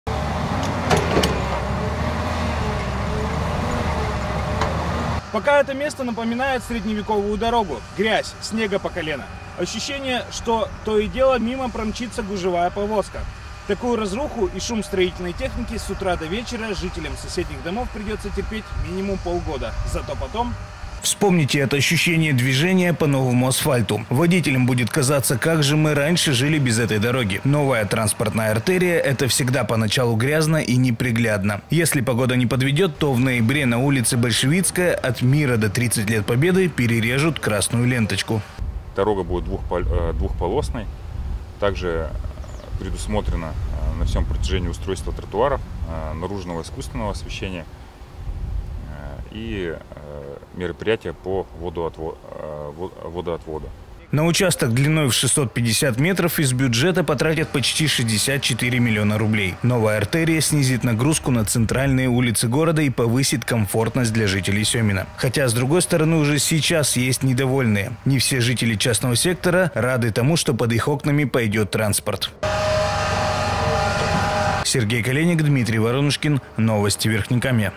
Строительство дороги
В Березниках в районе Набережной началось строительство автодороги от улицы Мира до 30 лет Победы. Чиновники предполагают, что таким образом смогут разгрузить улицу Пятилетки и создать для жителей Семино комфортные условия.